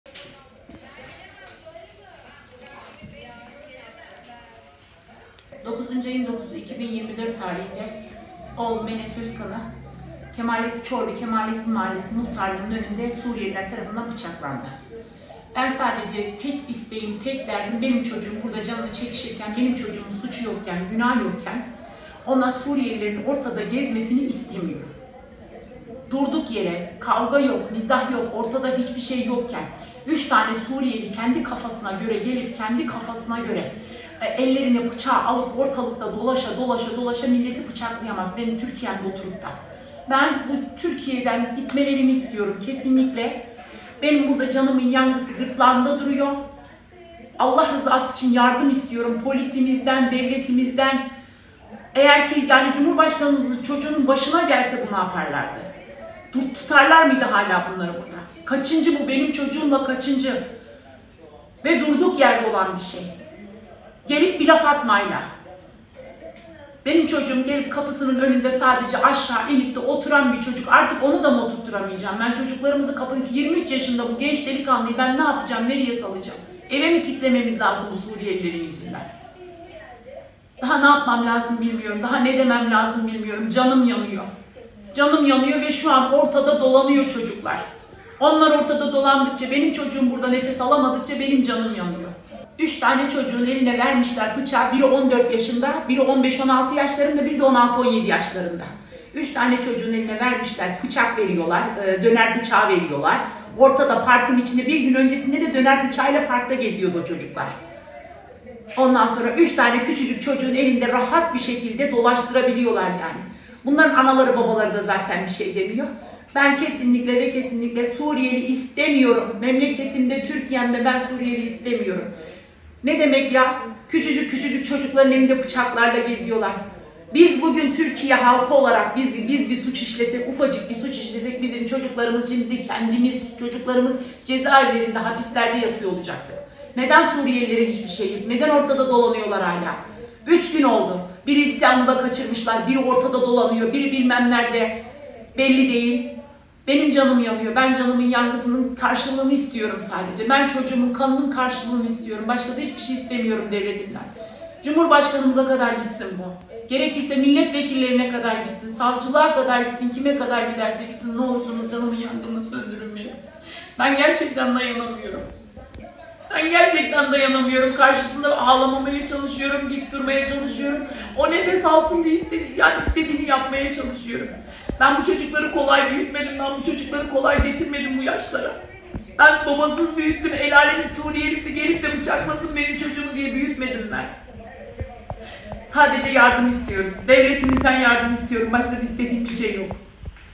GENÇ ENES YAŞAM MÜCADELESİ VERİYOR!
Çorlu’da geçtiğimiz gün Kemalettin Mahallesi Muhtarlığı önünde yaşanan bıçaklama olayı, mahallede büyük bir endişeye neden oldu. Yaralanan gencin ailesi ise suçluların tutuklanmadığını ve hatta kendilerini tehdit ettiğini iddia etti.